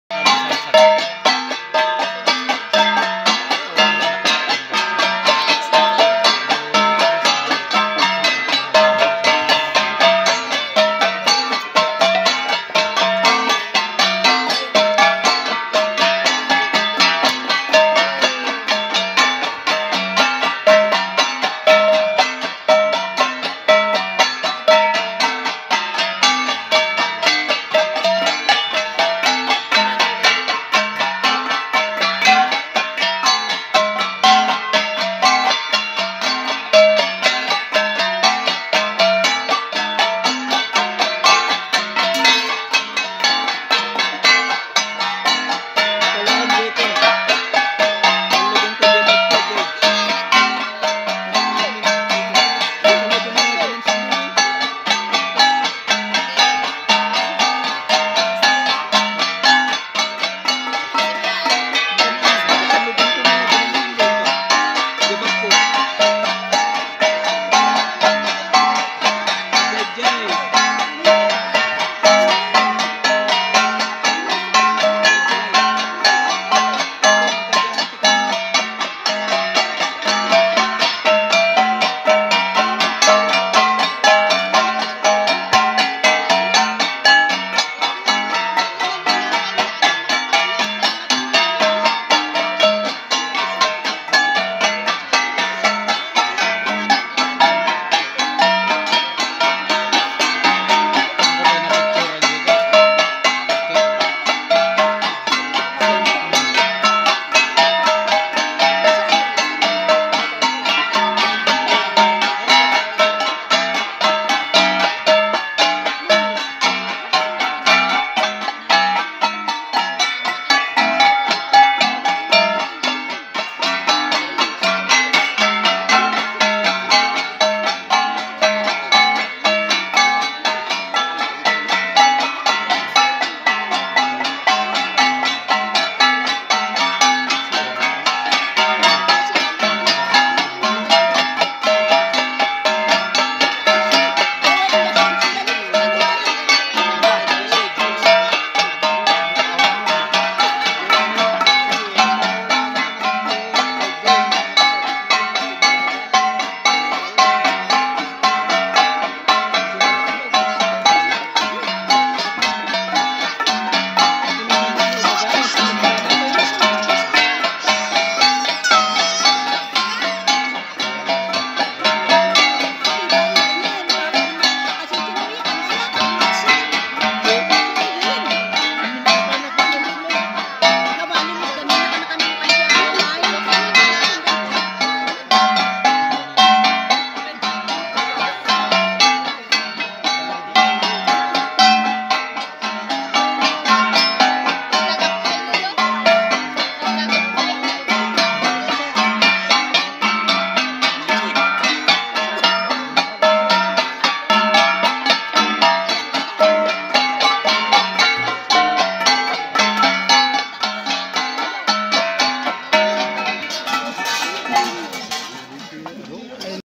Igorot Hunks Performs Pattong ( Igorot Wedding)
Group of men performs pattong at the wedding.

Thanks for the support and for watching my upload videos on my channel!

And Please Do Follow me on